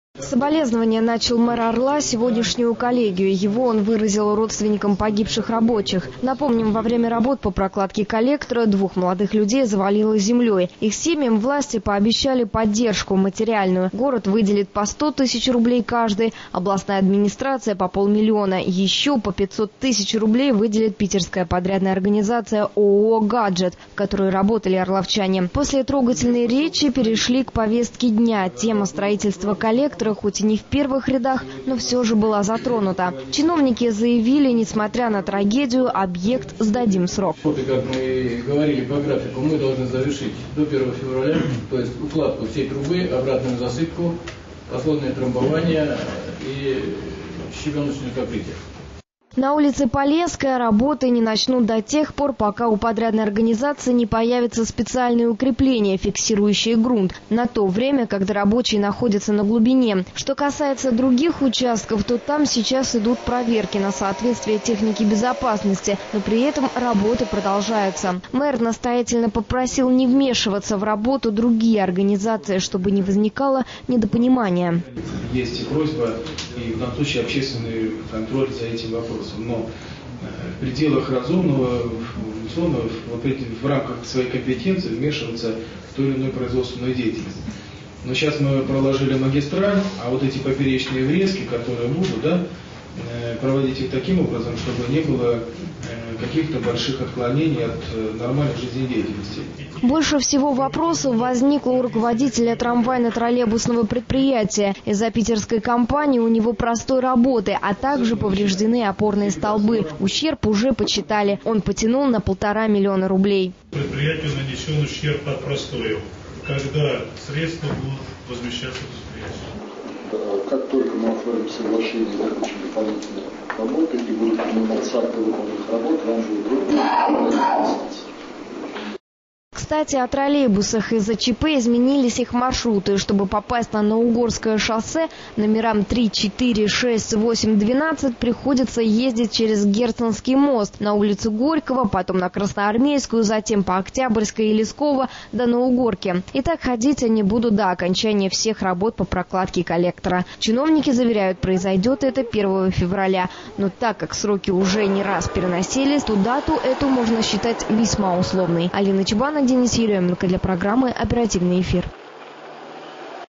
Истоки Орел trk-istoki.ru 17.01.2012
Несмотря на трагедию коллектор сдадут 1 февраля...(подробнее на сайте trk-istoki.ru)